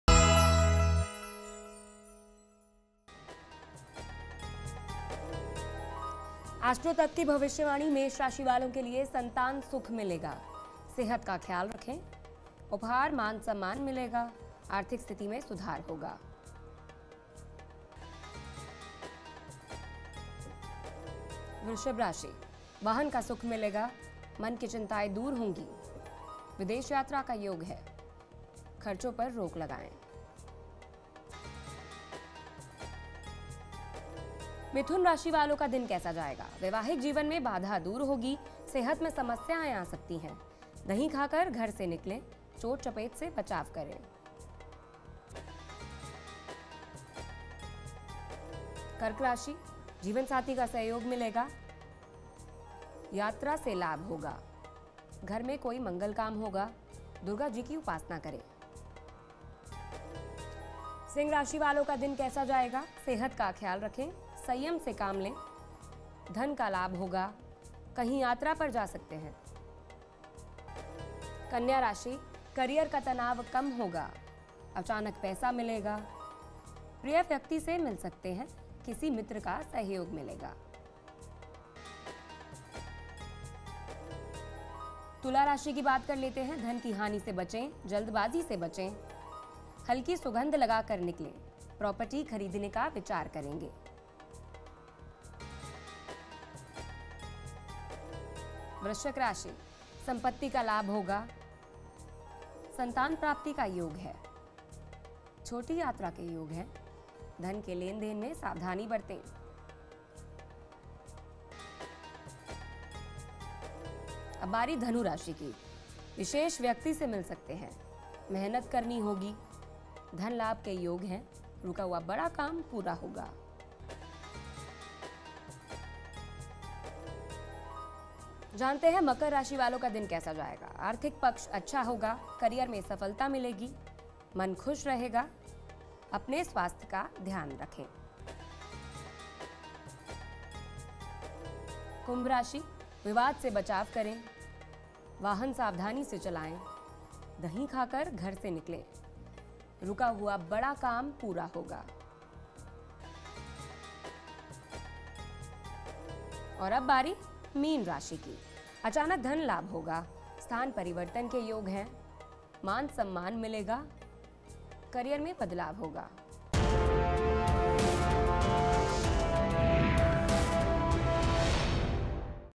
03 मई 2019 | एस्ट्रो तक का Luck Factor | Astro Tak
एस्ट्रो तक की भविष्यवाणी- 03 मई 2019- सिर्फ एक मिनट में जानिए अपने दिन का हाल और एक ऐसा उपाय जिससे दिन बन जाएगा कमाल AstroTak AstroTakkaLuckFactor (Ask a Question)